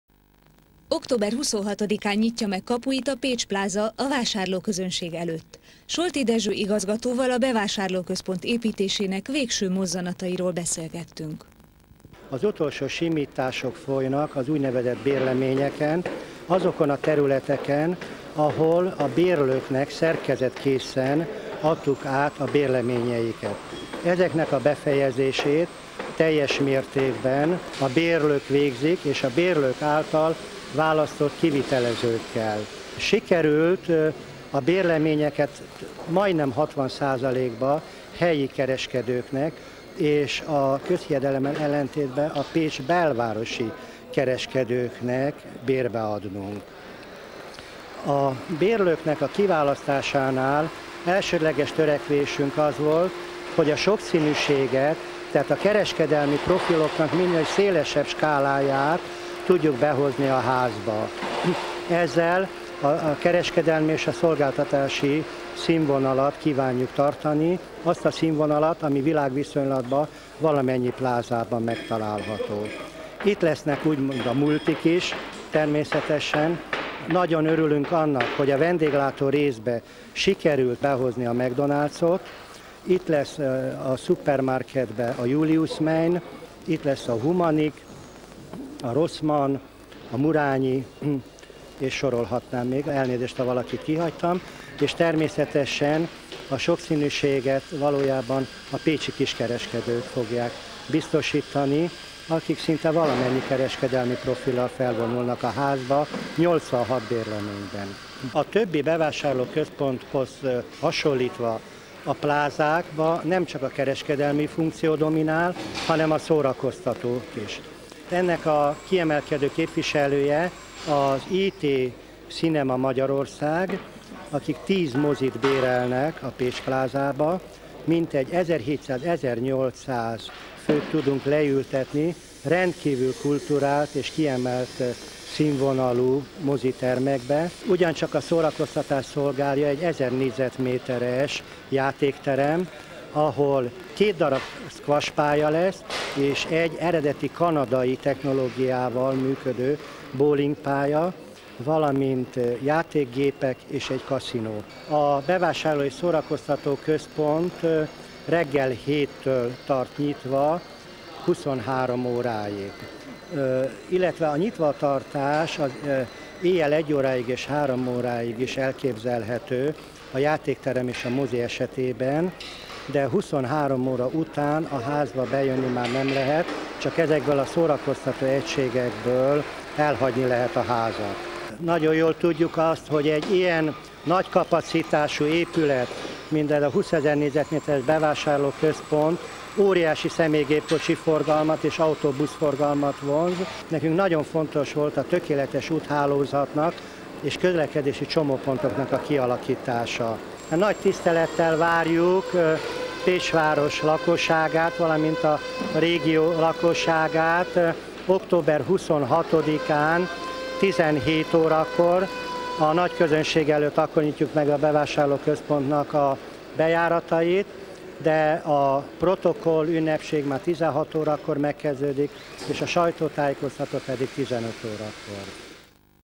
Pécs Pláza - Megnyitás előtti utolsó simítások (1999.10.13.) Pécs TV
Megnyitja kapuit a Pécs Pláza a vásárlóközönség előtt
Beszélgetés Solti Dezső igazgatóval, a bevásárló központ építésének végső mozzanatairól (0:12)

A filmfelvétel kizárólagos tulajdonosa a Pécs TV, felhasználója a Csorba Győző Könyvtár. A filmfelvétel sokszorosítása engedélyhez kötött!